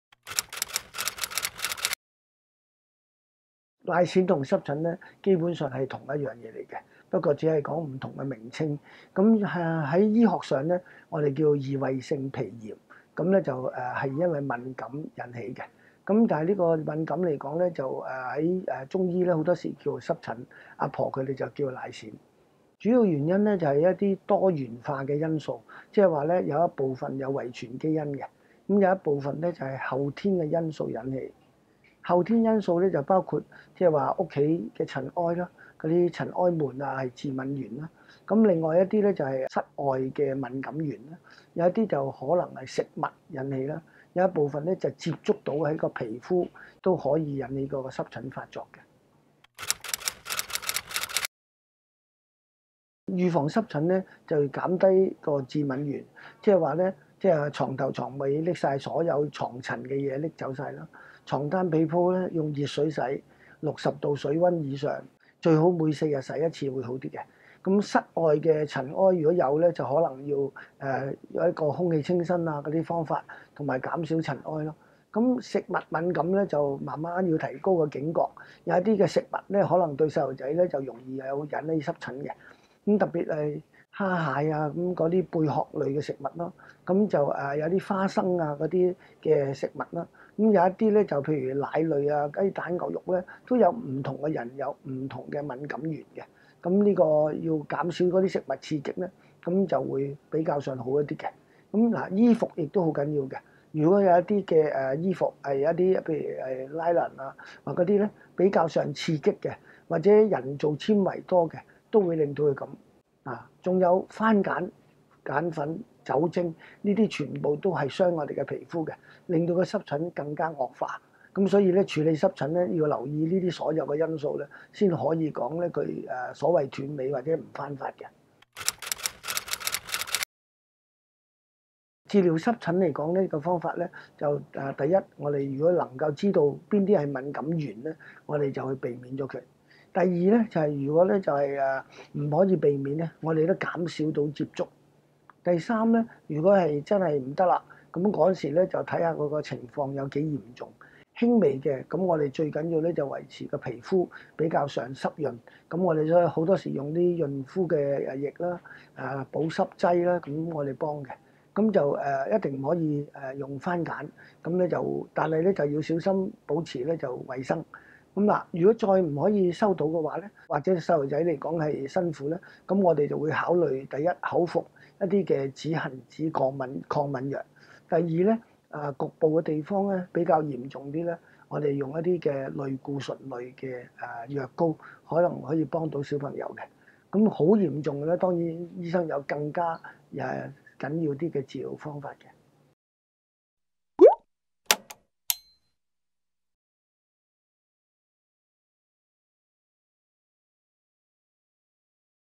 【專家話你知】BB奶癬
BB奶癬同濕疹其實係一樣病症，引起原因分為遺傳基因同後天因素，如果後天因素控制得好，咁濕疹就無咁容易復發喇！一齊嚟聽下趙長成醫生點講。